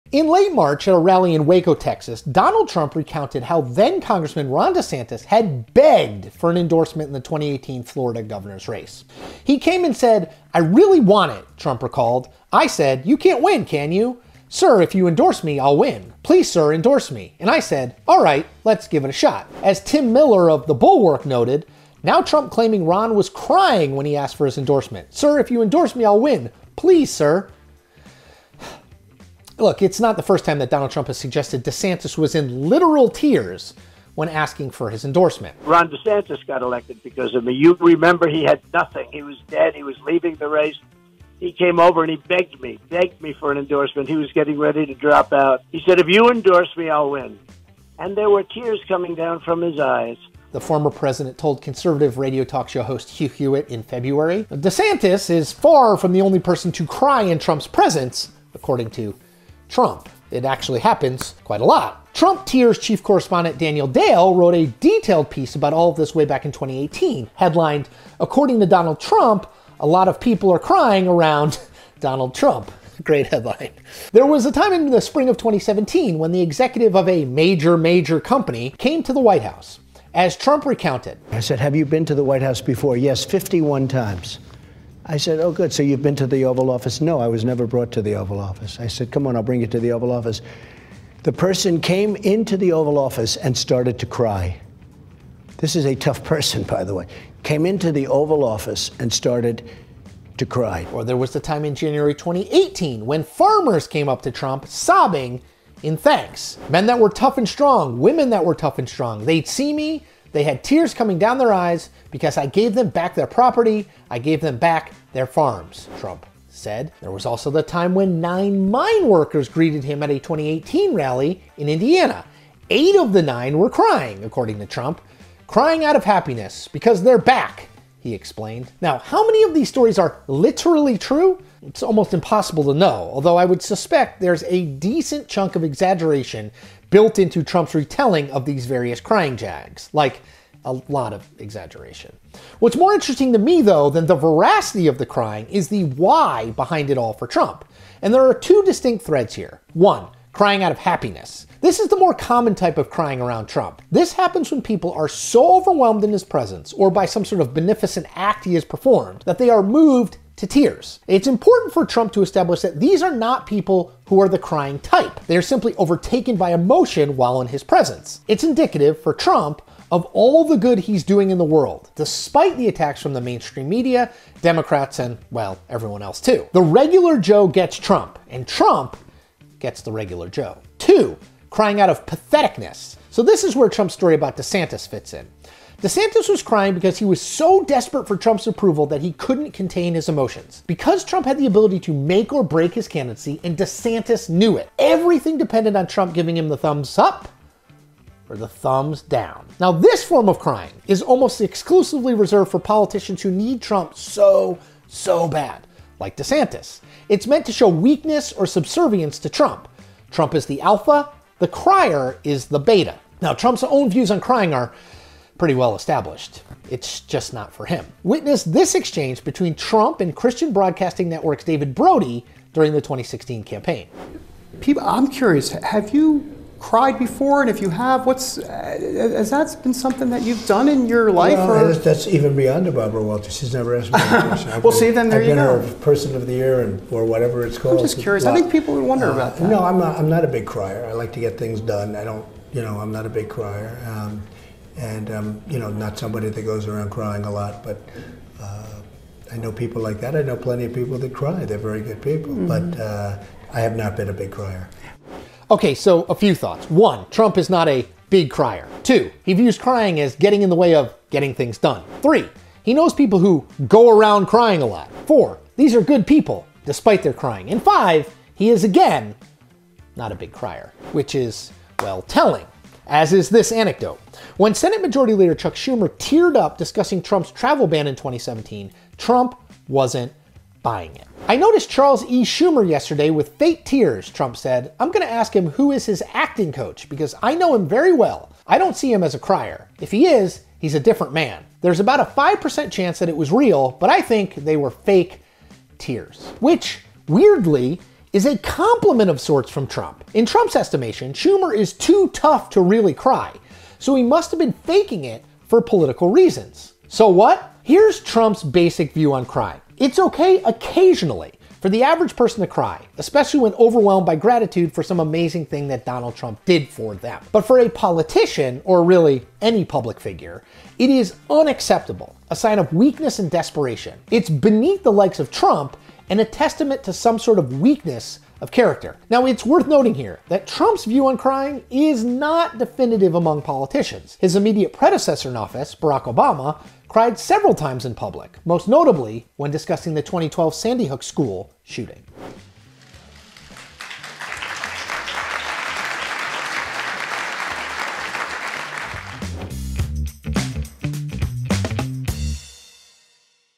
Donald Trump's crying obsession
Lots and lots of people cry around Donald Trump, according to Donald Trump.